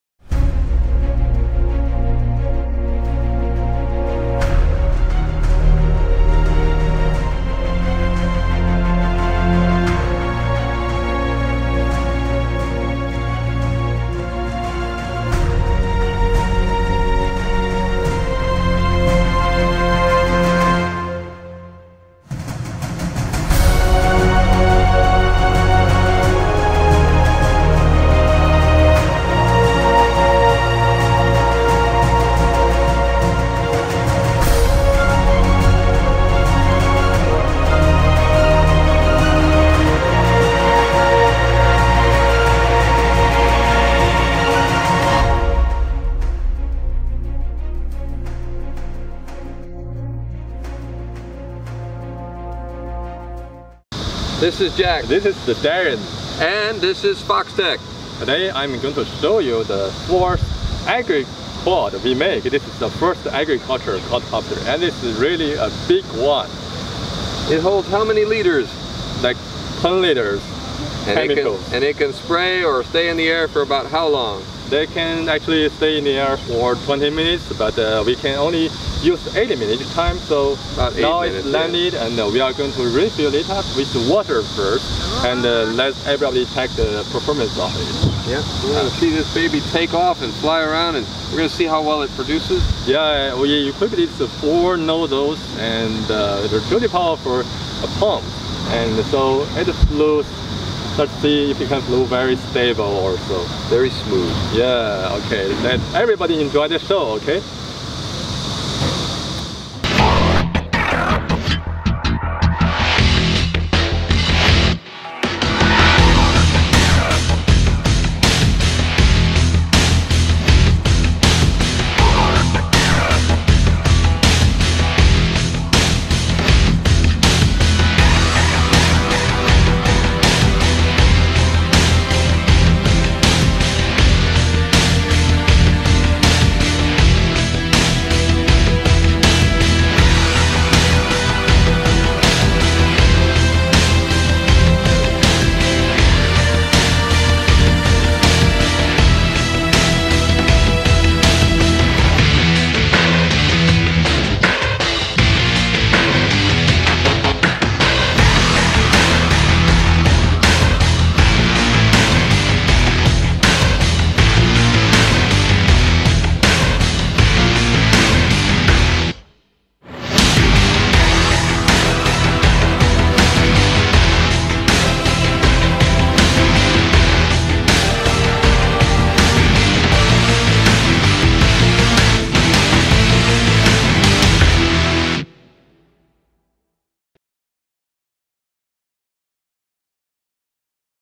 Foxtech A4-1 Crop Sprayer Quadcopter
A4-1 is the latest product developed by Foxtech  as a crop sprayer.
Descriptions:
Wheel Base: 1200mm
Drone Weight: 6.9KG (no battery)
Container Volume: 10L
Single Operation time: 15mins
Spray Volume: 800-1400ml/min
Sprinkling Width: 3m
Spray Coverage: 14-25 acres/hour

will be available for sale soon.